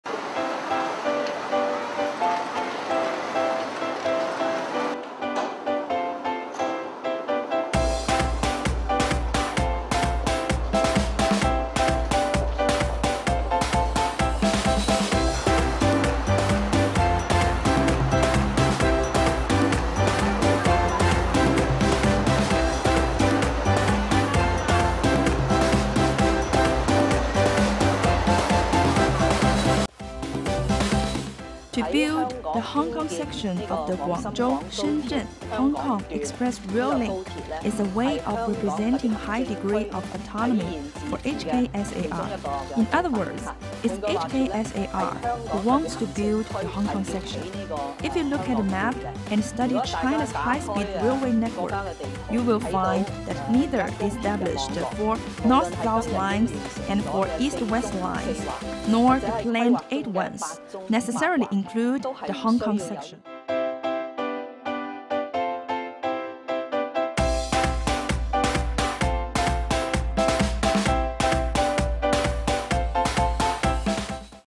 HKSAR Chief Executive endorse Hong Kong section of Guangzhou-Shenzhen-Hong Kong Express Rail Link
Can people work in one city and sleep in another? In Guangdong-Hong Kong-Macao Greater Bay Area, it could be possible. Find out how HKSAR Chief Executive endorse it.